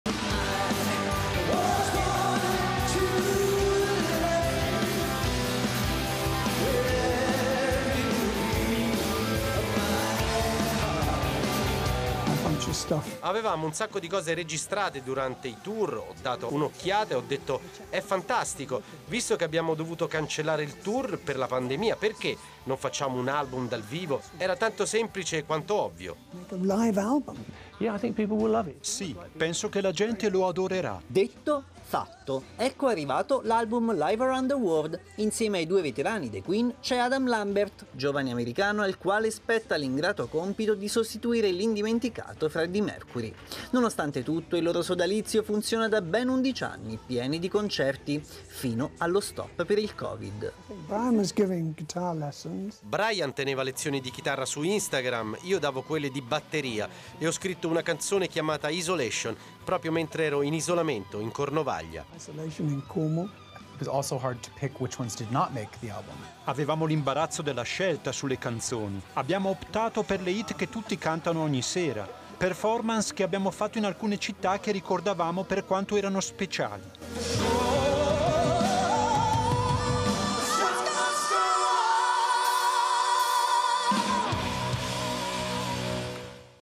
“Queen” pubblicano album con i migliori concerti a 29 anni dalla morte di Freddie Mercury
A 29 anni dalla morte di Freddie Mercury, i Queen, Brian May e Roger Taylor, pubblicano l'album che raccoglie i migliori momenti dei loro concerti. Il servizio di Silvio Vitelli